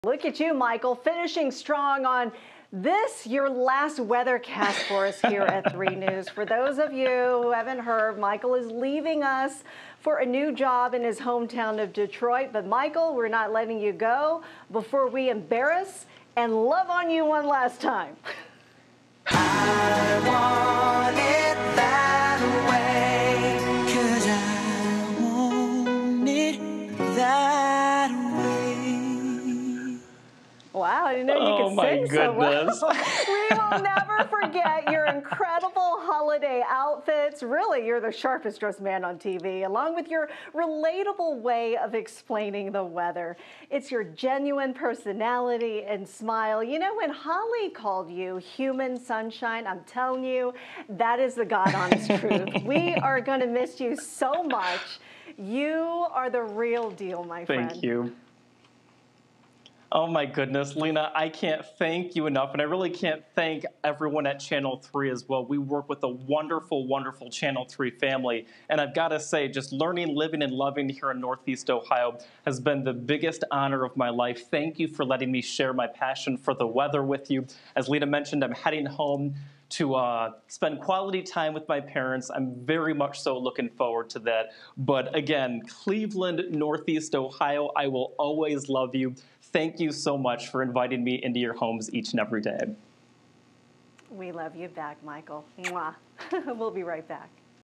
Lynna Lai bids a fond farewell to Michael Estime
Lynna Lai pays loving tribute to Michael Estime. Our smiling forecaster is heading home to Detroit.